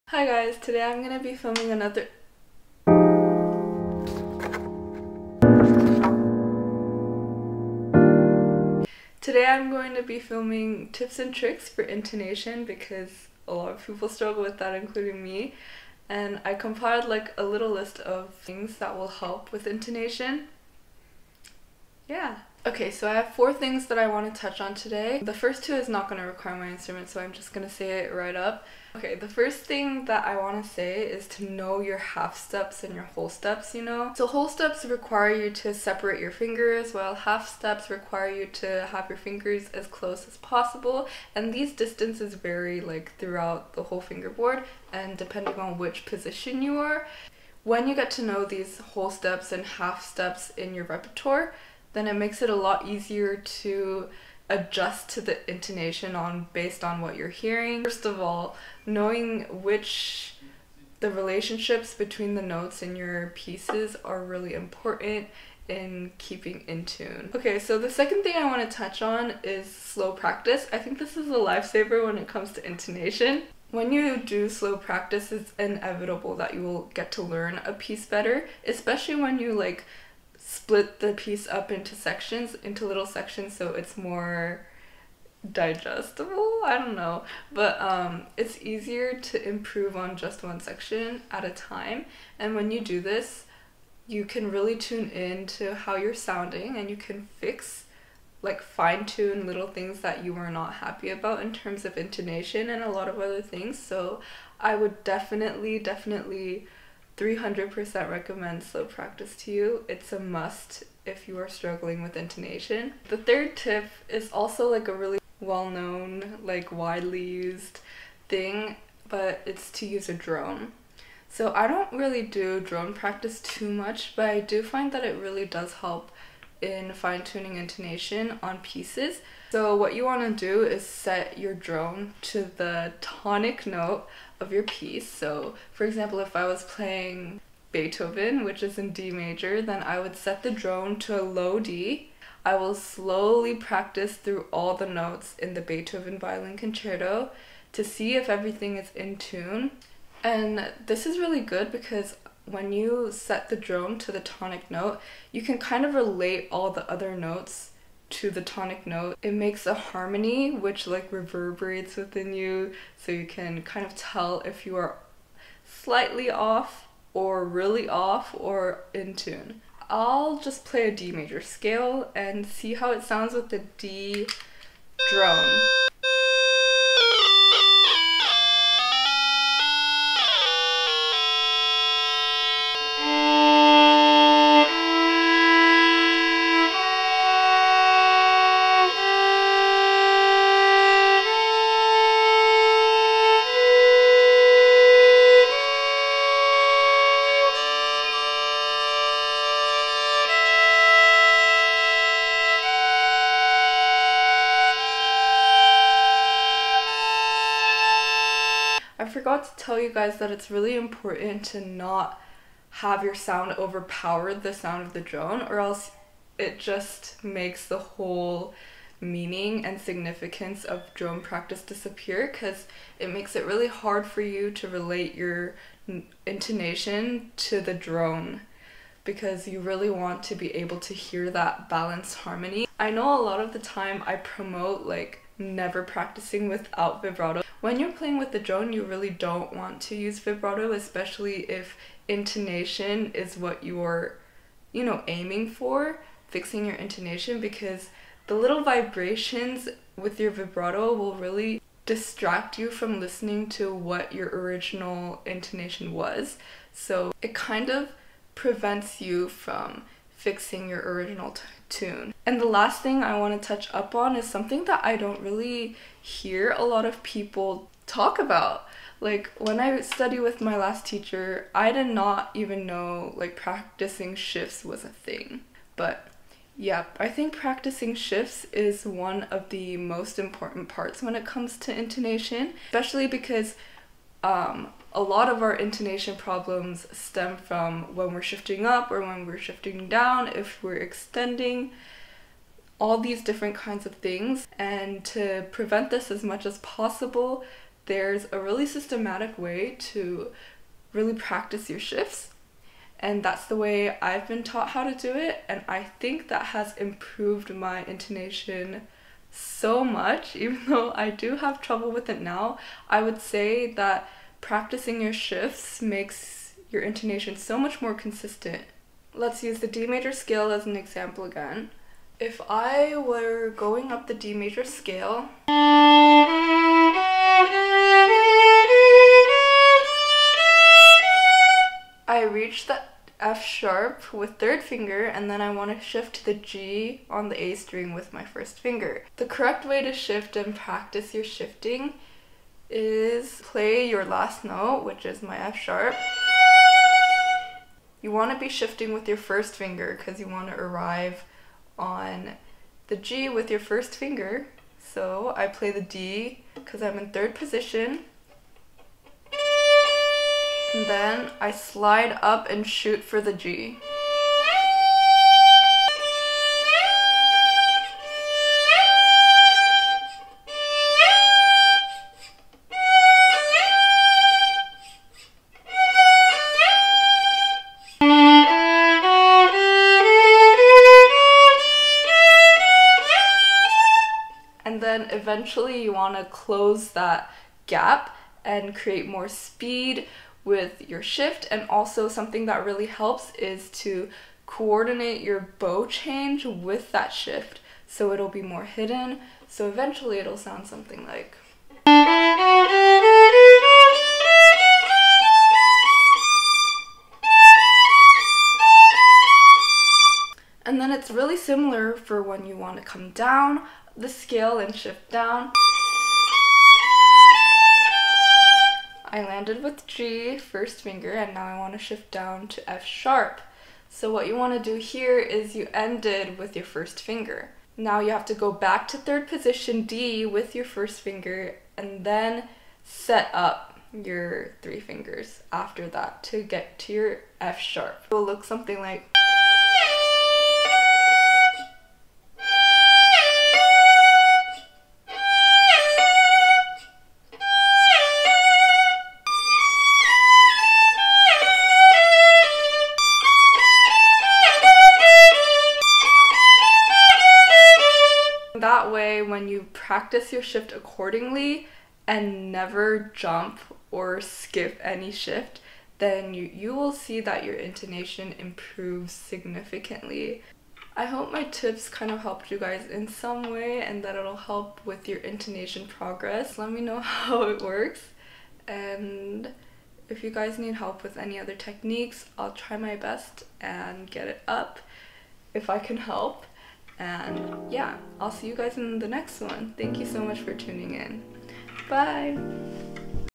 HOW TO HAVE BETTER INTONATION- Violin Help ep. 5
Hey guys!! It’s been a long time since my last violin help video but here’s another one. I think intonation is something that even the best players struggle with so I wanted to talk about some things that made it better for me! I understand that everyone has different ways of practice that will work best for them but I hope some of these REALLY click with you guys. Remember that consistency is KEY.
Thank you for tuning into this video and ALWAYS let me know if you guys need any help. Thank you!!

Like and sub and comment!!🥰